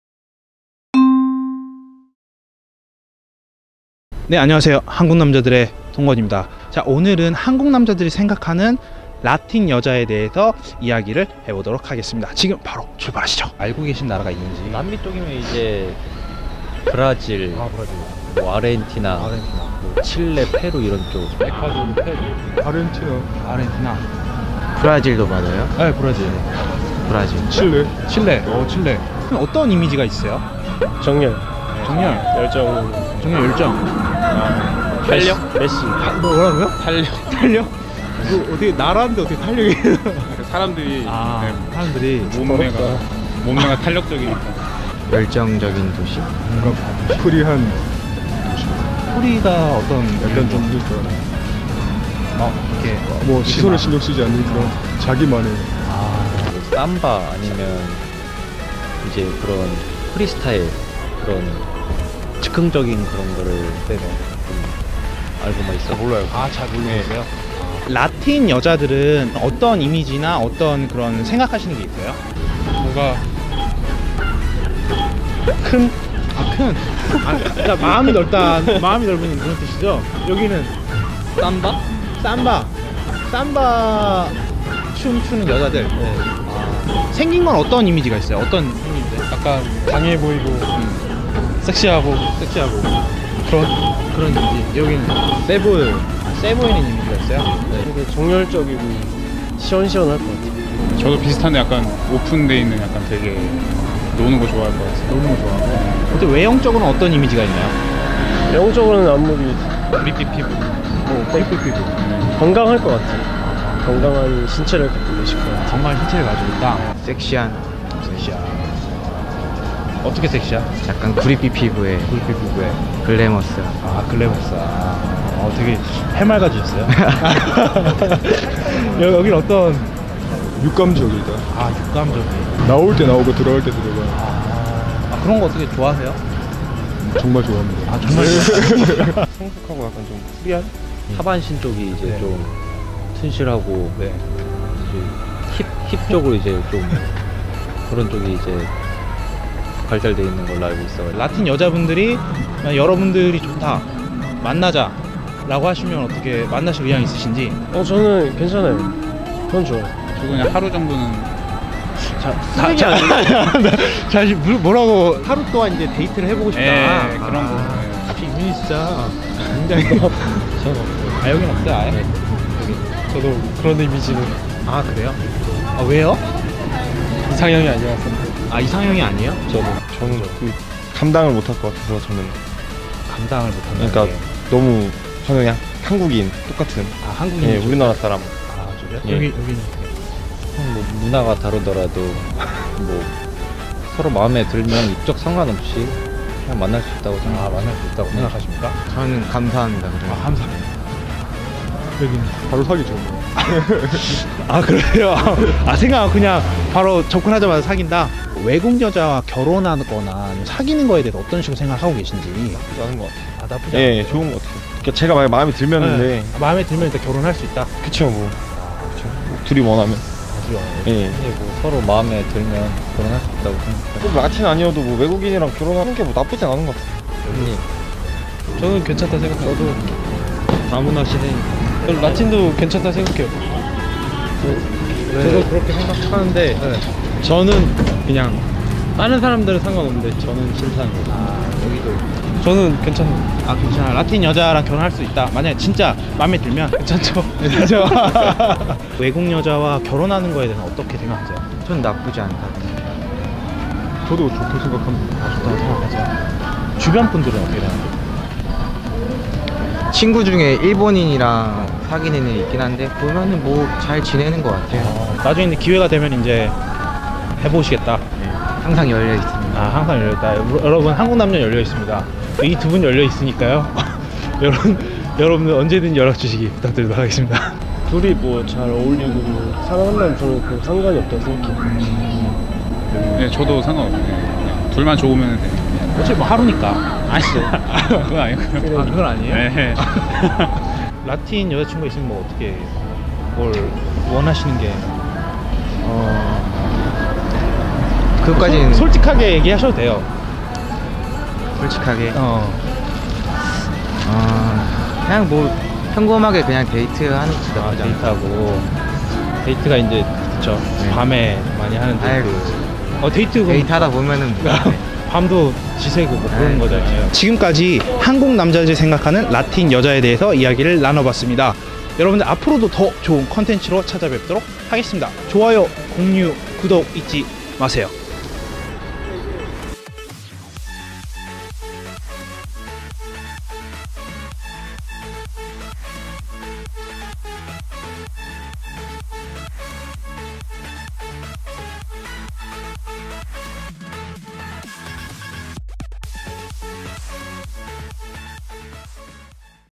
Lo que coreanos piensan de las latinas | Korean guys
Lo que coreanos piensan de las latinas. Como piensan los chicos coreanos de las latinas!
Que piensas de Latinoamerica?
Si las chicas latinas te dicen que les gustas y ...Ellas quieren salir contigo, Estar?s dispuesto a considerarlo?